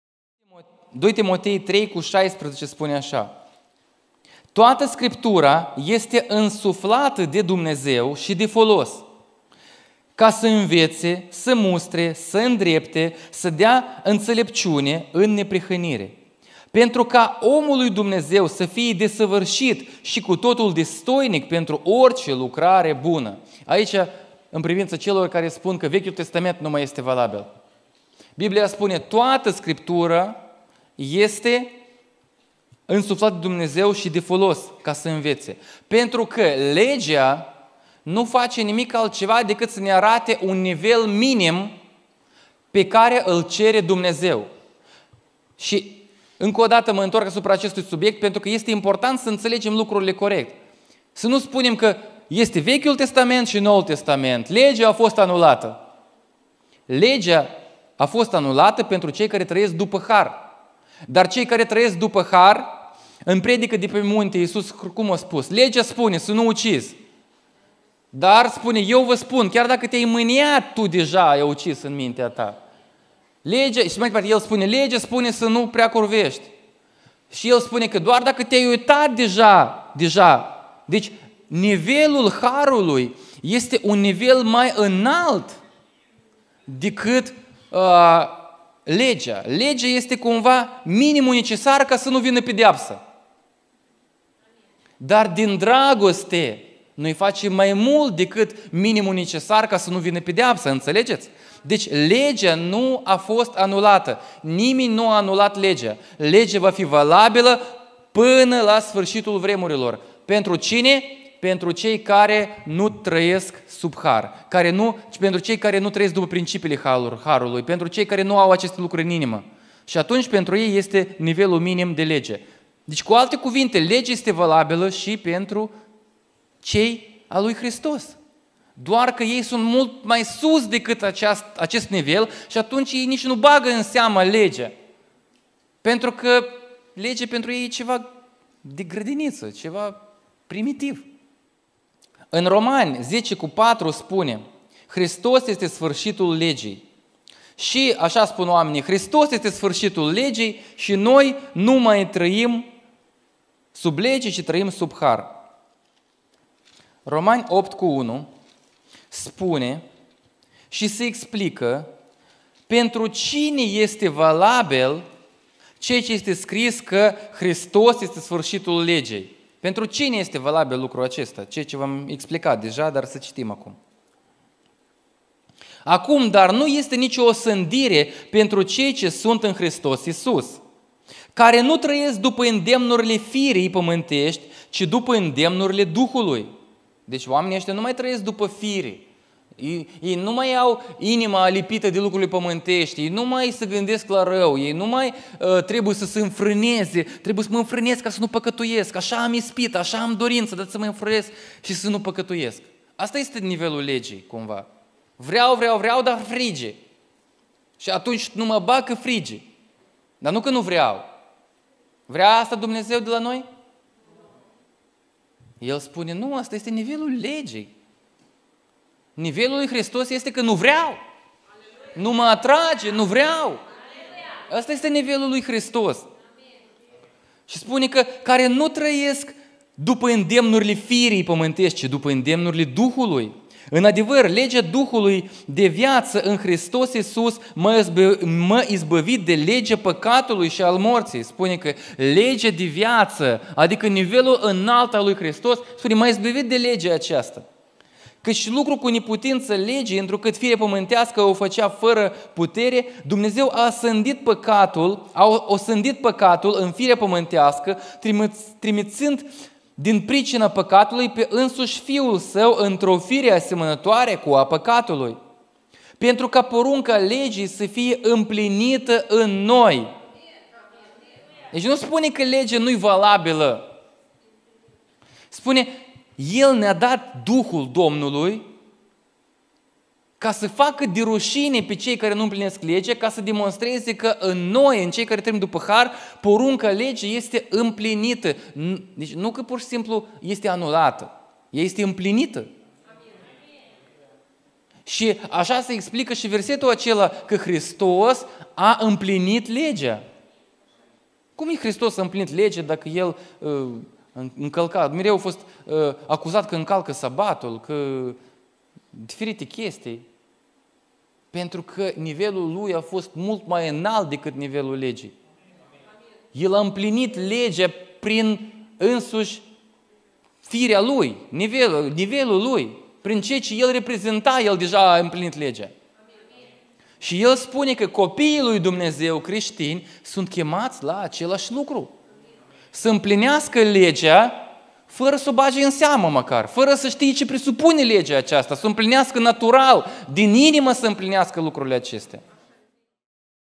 HARUL din VIAȚA TA
Legea lui Dumnezeu nu a fost anulată de Isus Hristos. Toată Scriptura este de folos ca să înveţe. Legea este nivelul minim pe care îl cere Dumnezeu. Isus a adus un nivel mult mai înalt, trăirea prin har.